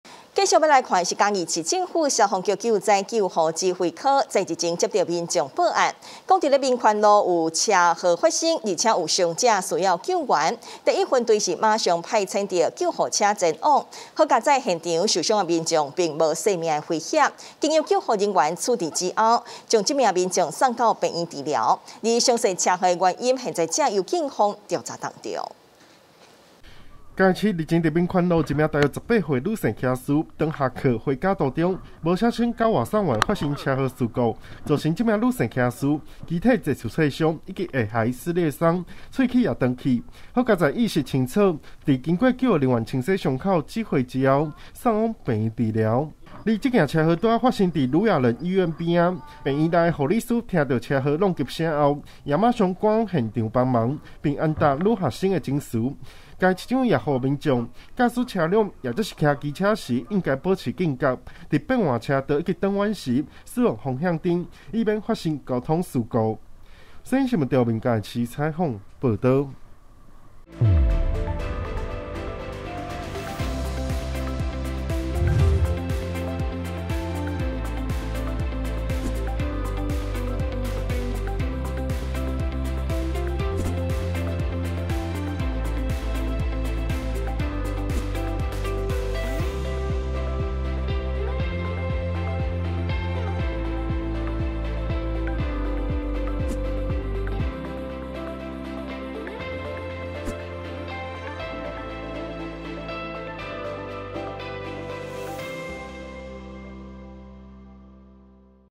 世新新聞 騎士未注意與外送員擦撞門面受傷送醫
110年09月23日 (四) 世新新聞

騎士未注意與外送員擦撞門面受傷送醫

嘉義市政府消防局救災救護指揮科接獲民眾報案表示，民權路有車禍傷者急需救護，第一分隊馬上派遣救護車前往救援。所幸現場傷者無生命危險，再經救護人員處治之後，將傷者送往醫院治療，詳細車禍原因由警方調查當中。